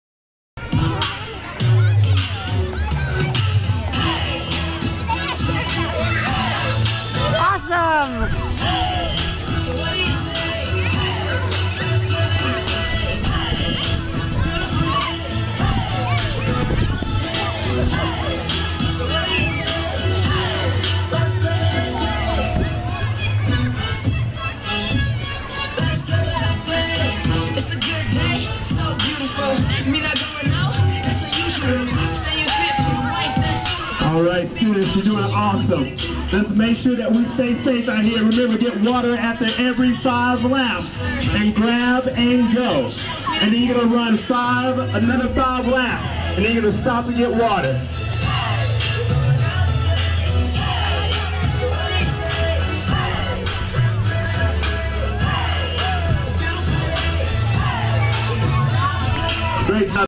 Rachel's Minute 2271 - Oct 4, 2012
Rachel participates in her school fun run